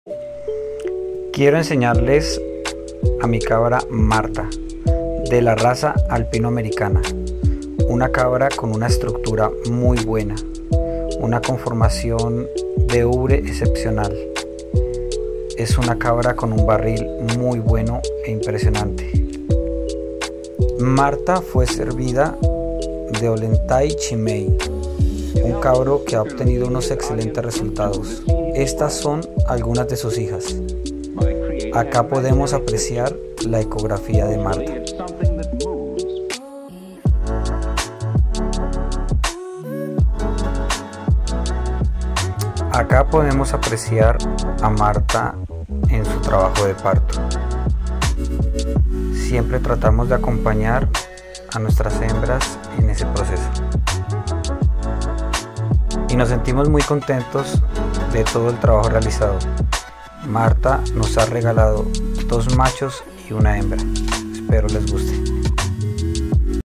cabra de raza alpino americano
🚨🚨Nuestra cabra MARTHA😍, de la raza alpino americano🐏, está hembra es impresionante💎, muy lechera🥛 y estructurada, siempre buscamos mejorar🧬 por eso la servimos con 🥇🤴🏼OLENTAGY OAA CHIMAY, con semen congelado✈️ ( pajilla), un cabro que tiene hijas impresionantes😍😍, muy lecheras y de estructuras  muy buenas, está chica nos regaló 3🫣 hijitos,  una hembra y 2 machos, 🧬🥛que tienen un valor muy alto🏔️ para seguir mejorando nuestro trabajo💪

Quieres conocer👀 mas sobre nuestras cabras, te invitamos🫱🏻‍🫲🏻 a que nos sigas en nuestras redes sociales 🔥para conocer mas de esta bella especie 😍y algunos tips que te pondrán servir en el día a día💪🏻

Canales